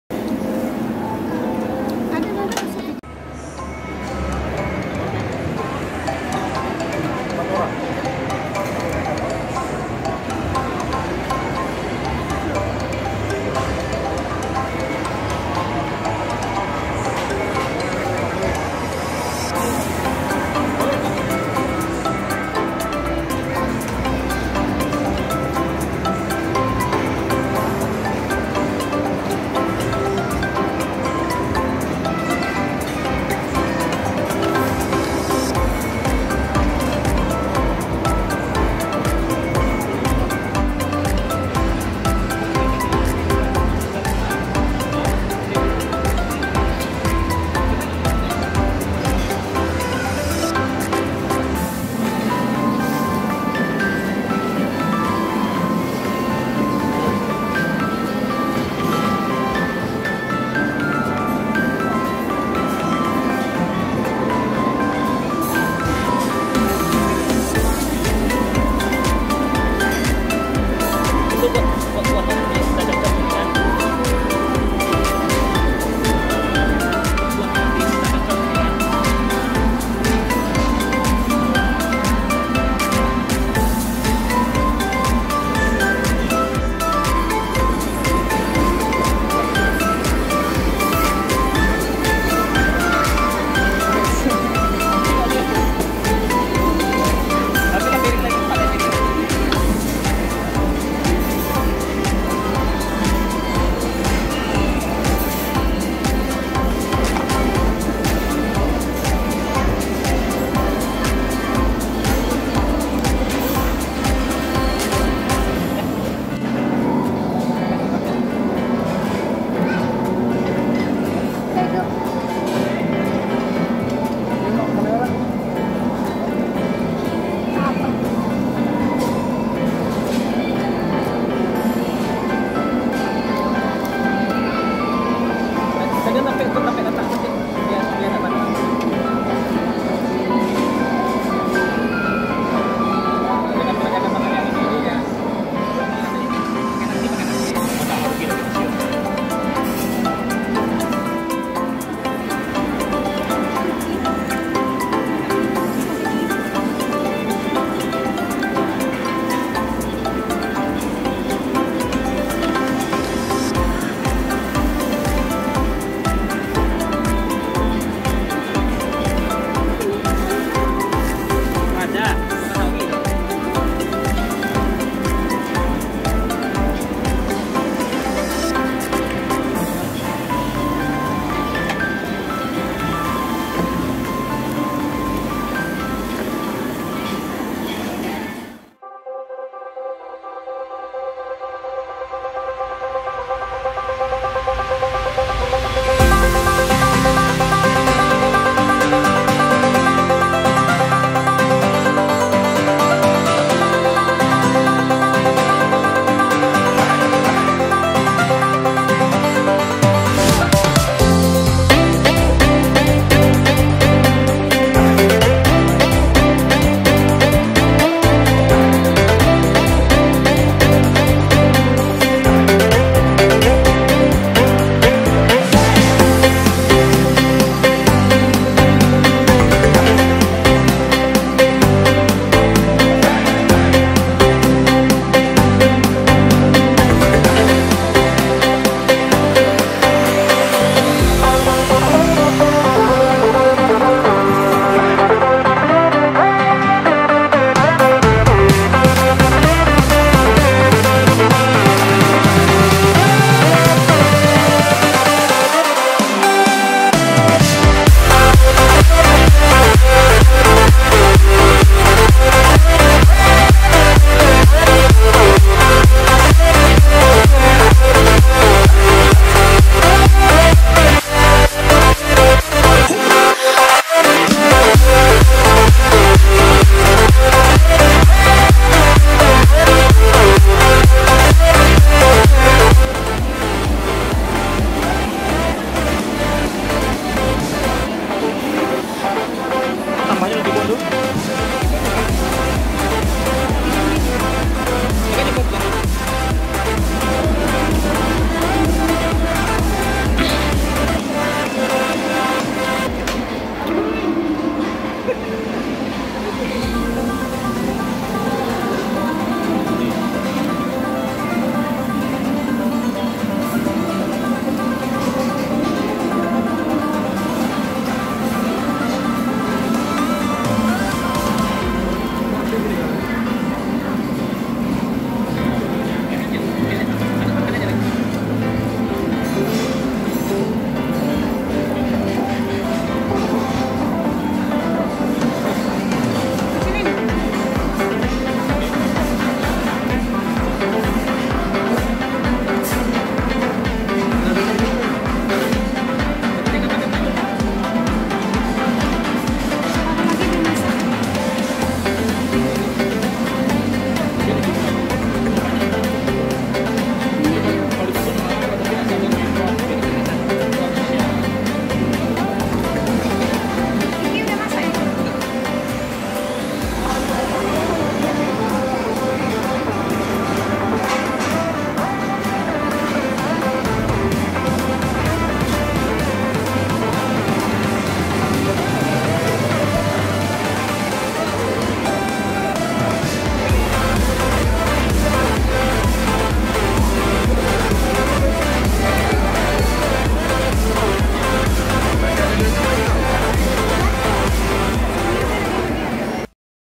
All You Can Eat Japanese BBQ
Coba makan di All U can eat, BBQ Batam.  Harga sesuai dengan rasa dan pelayanan. Pengen balik dan makan disini lagi..